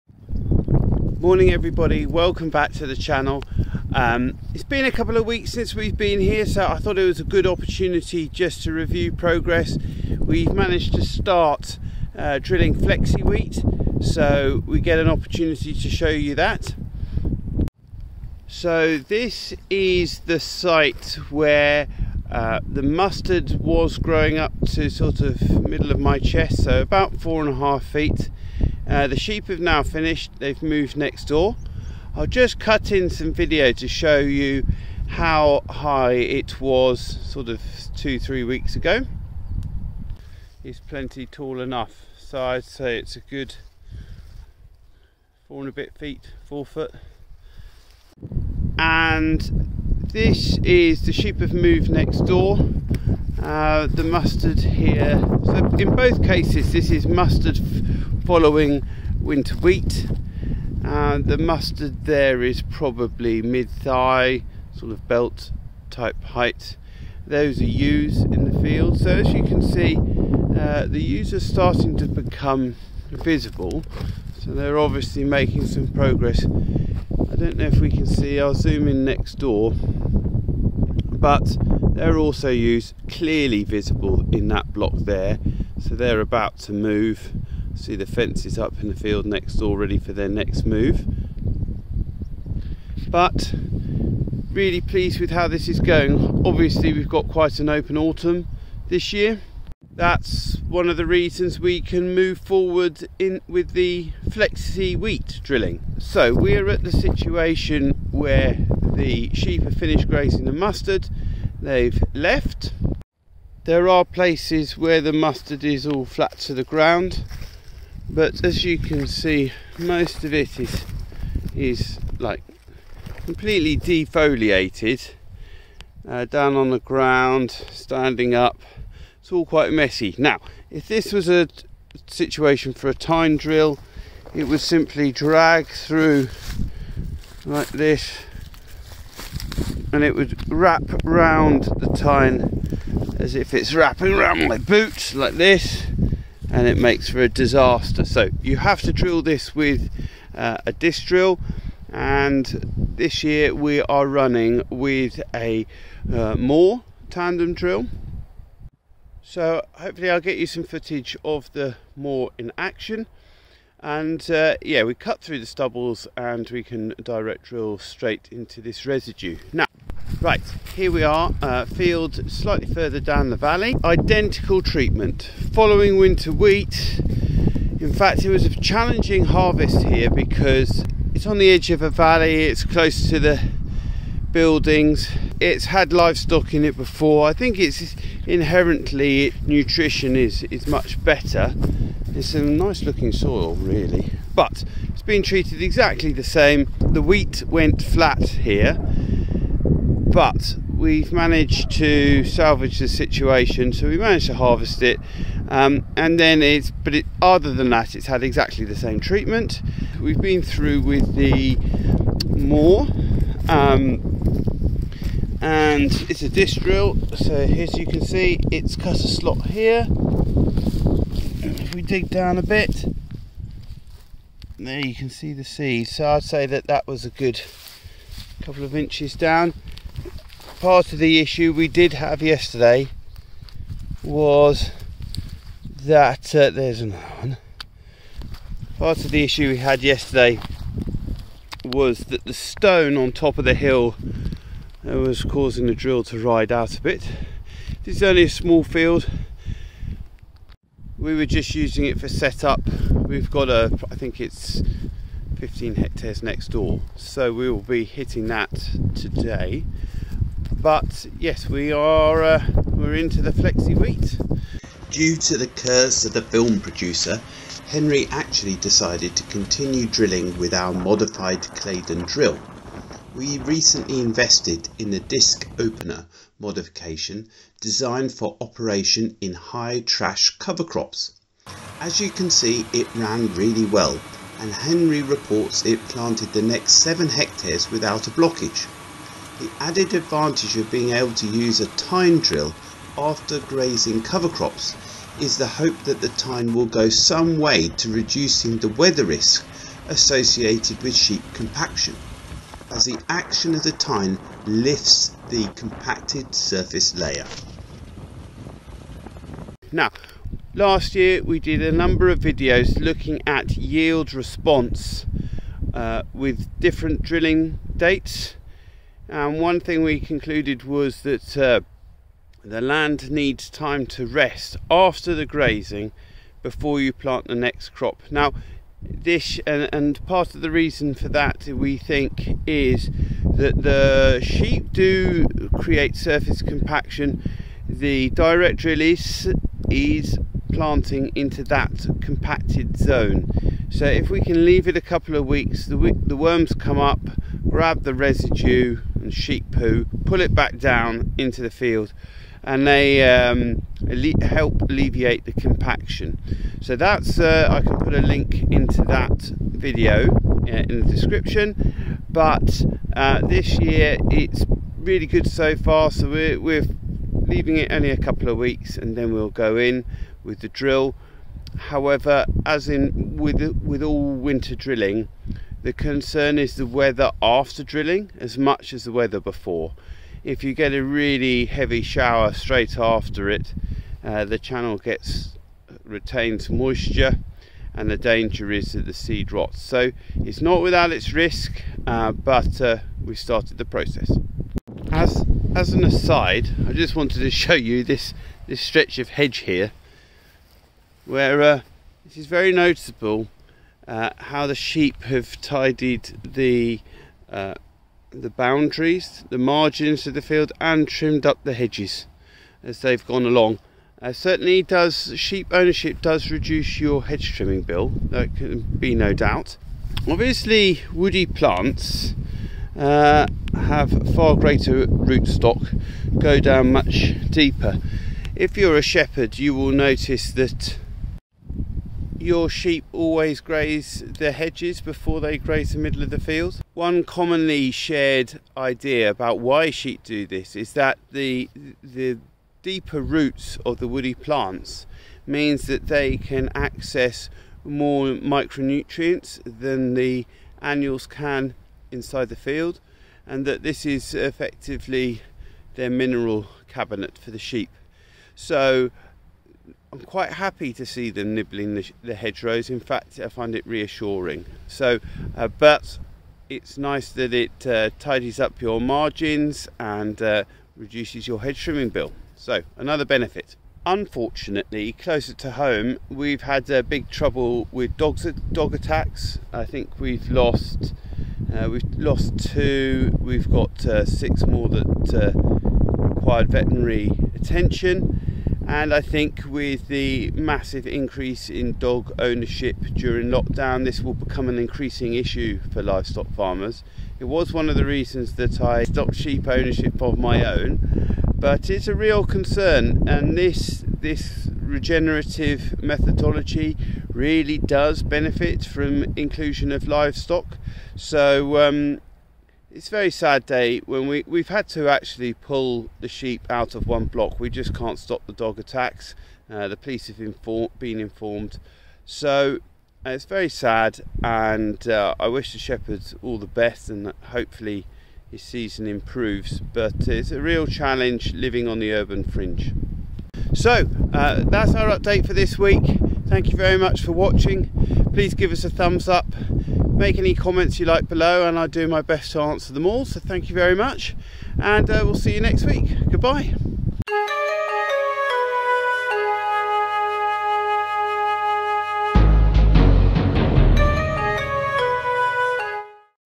Planting Flexi wheat
This week we start planting flexi wheat into mustard cover crop residue.